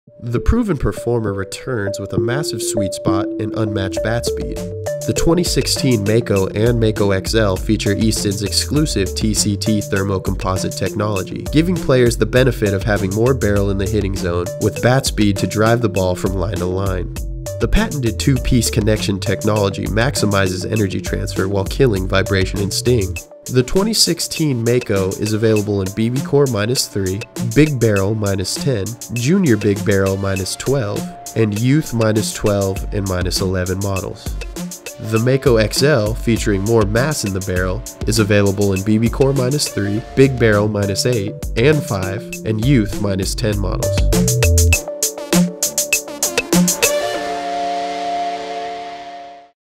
Easton - Mako & Mako XL Baseball Bat Tech Video (2016)
The 2016 Mako & Mako XL return with massive sweet spots and unmatched bat speed at the plate. These proven performers, featuring TCT™ composite technology and Easton’s patented two-piece ConneXion technology, will give you more barrel in the zone with the ability to drive the ball from line to line. Available in BBCOR, Big Barrel and Youth models.